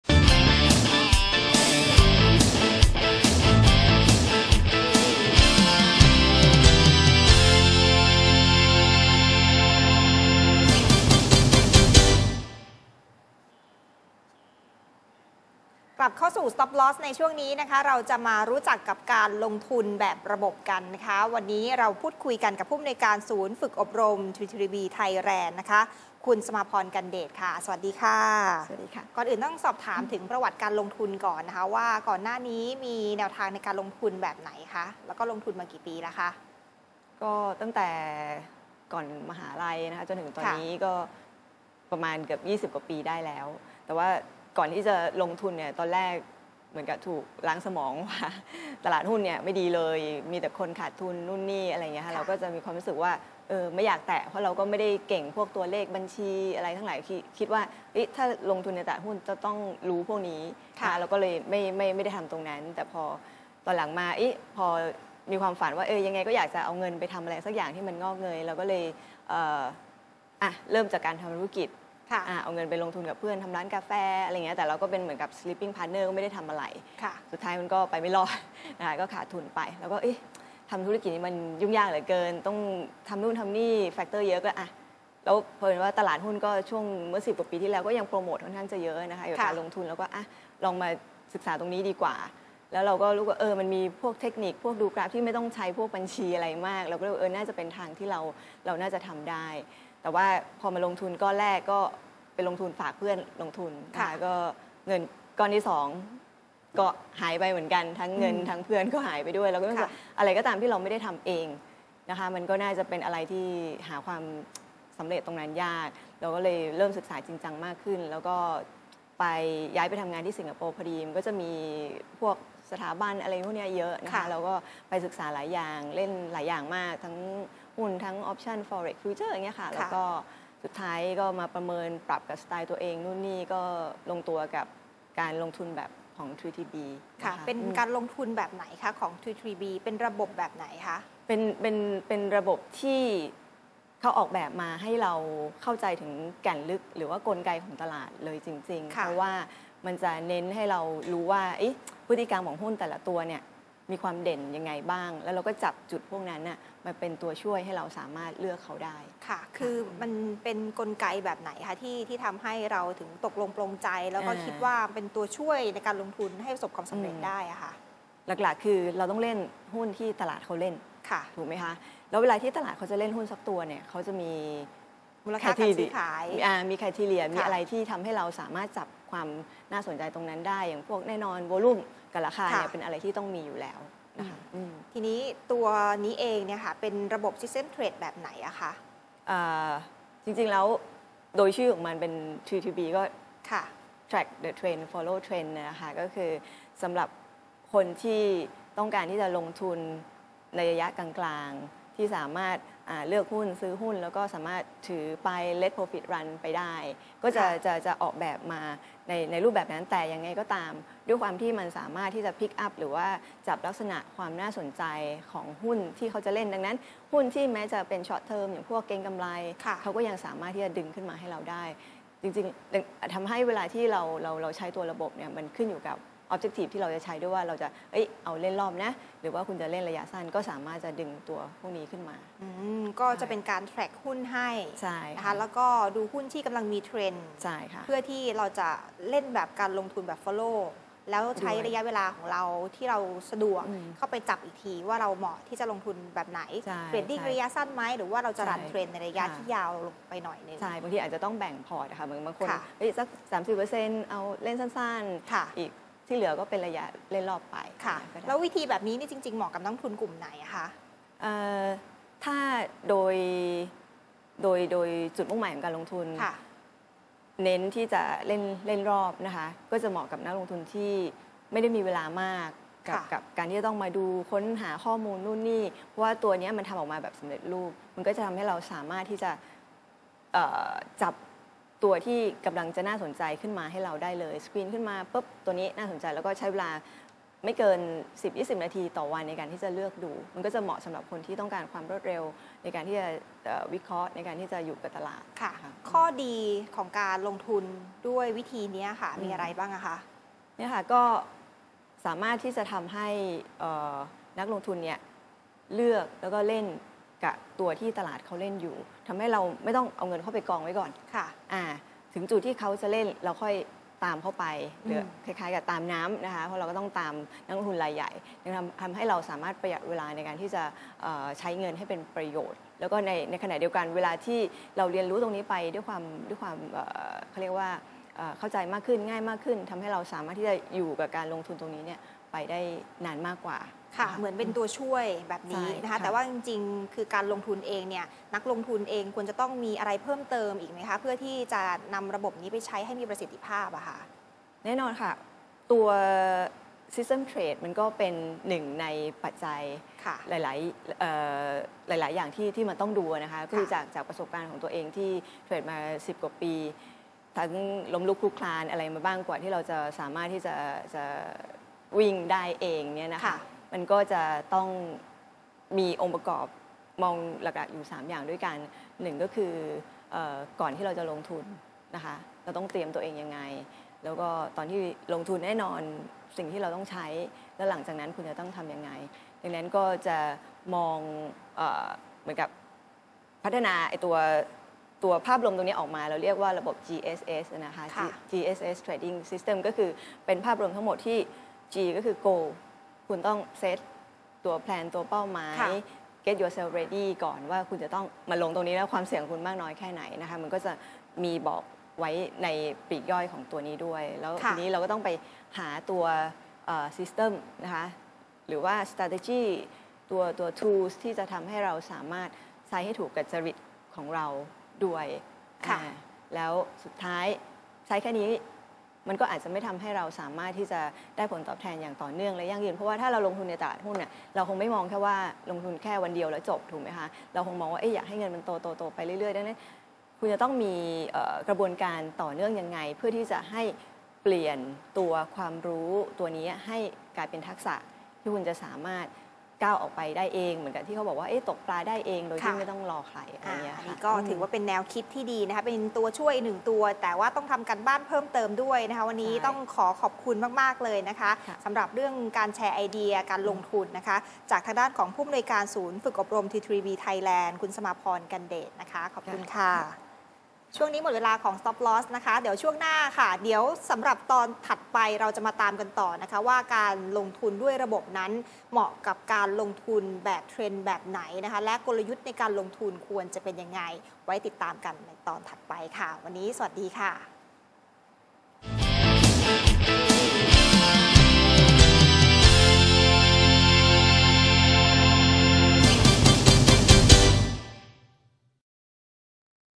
6-5-58 Stop Loss 16.30 T3B_1
"วิเคราะห์หุ้นด้วยระบบ T3B" กับคุณสมาพร กันเดช  ผู้อำนวยการ บริษัท ที3บี (ประเทศไทย) จำกัด ผู้บริหาร T3B Training Center ศูนย์อบรมความรู้เรื่องหุ้น จากประเทศสิงคโปร์